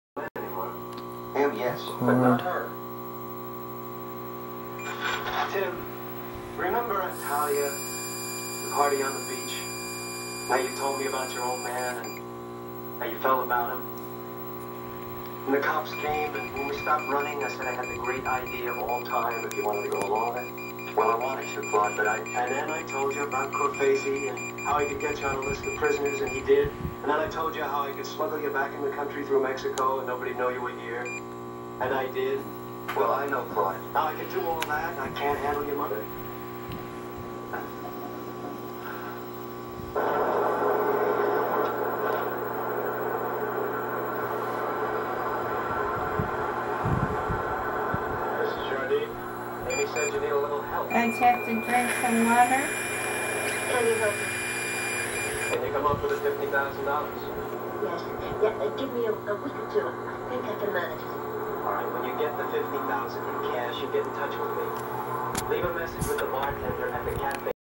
How do I reduce my BAC?
Jeffuehrer demonstrates how to reduce your blood alcohol concentration test from a .118 to a .000.